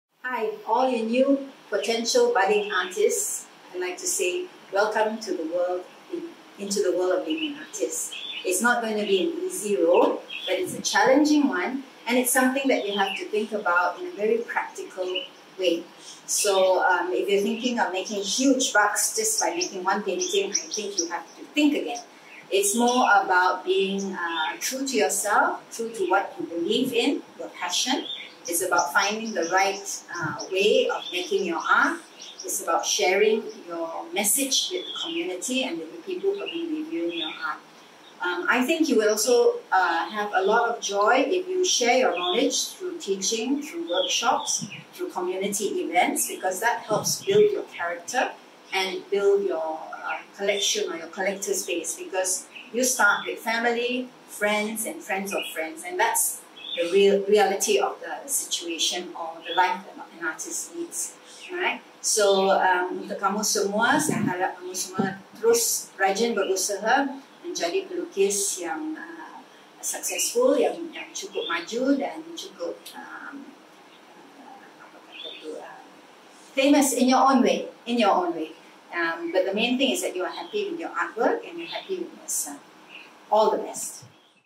Christianne Goonting's Advice to Visual Arts Students
This is a video advice by Christianne Goonting, a well known artist in Sabah, to Visual Arts students about how the practical life of the artist is and what to expect.

This advice is a part of an interview with her on Nov 7, 2019.